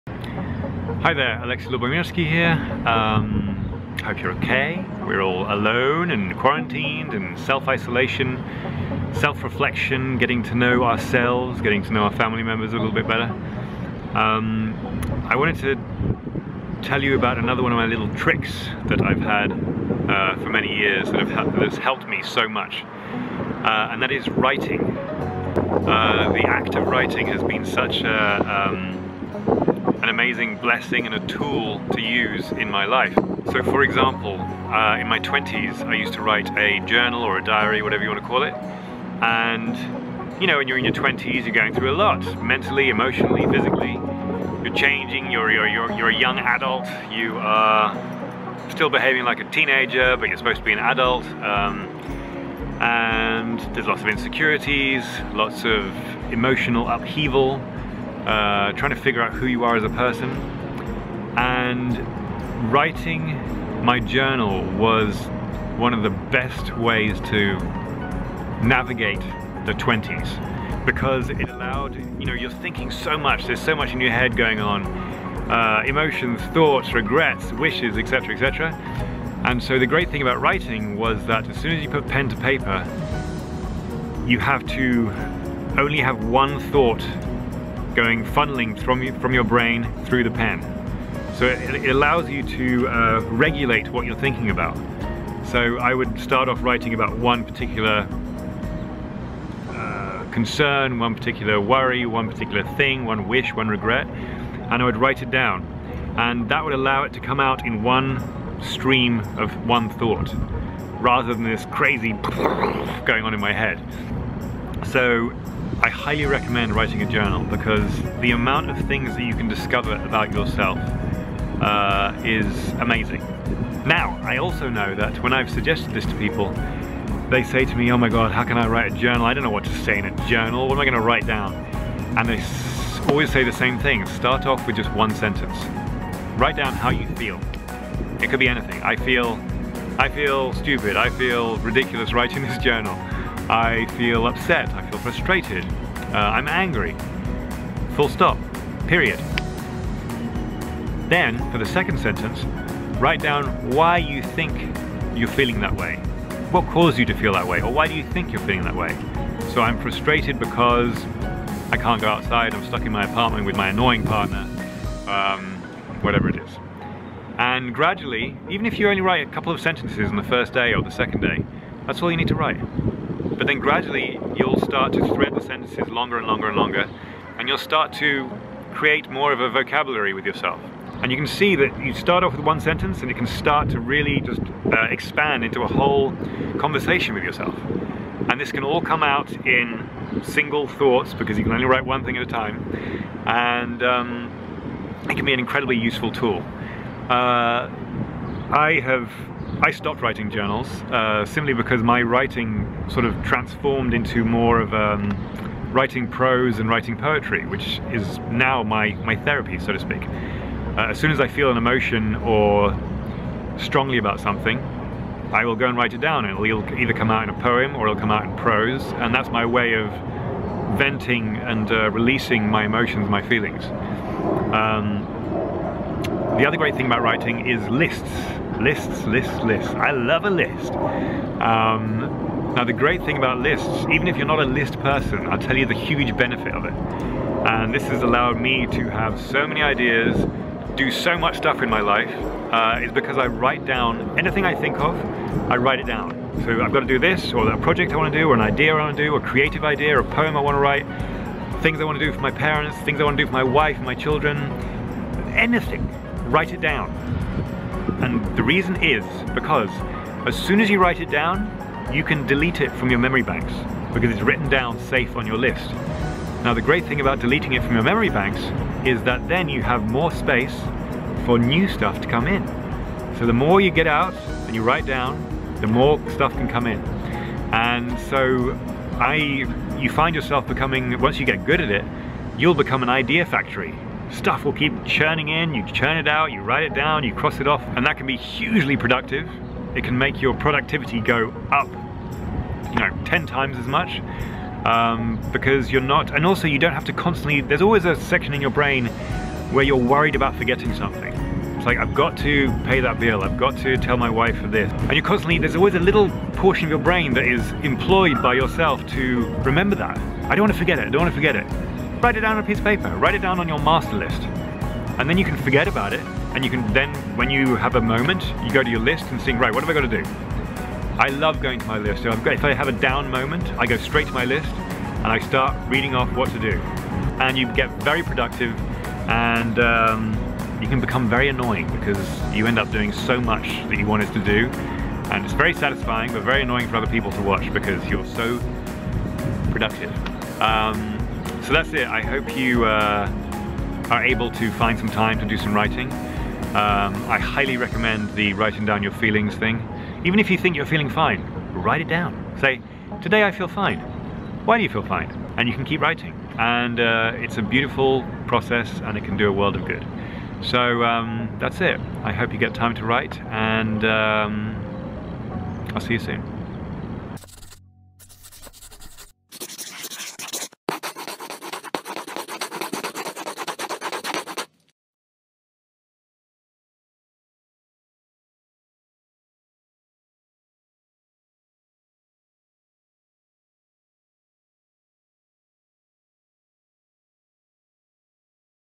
The Power of Writing - Life Hack for Quarantine
I'm back with yet another life hack for you guys! During this time I am working to better myself so I can come out of this time a bit more evolved. Over the years I have created my own database of life hacks that have helped me in times of reflection and I am honored to share them all with you! This time it is writing, writing has helped me evolve and reflect in more ways than one. Enjoy!

Please let me know if you have any questions or if you tried this out!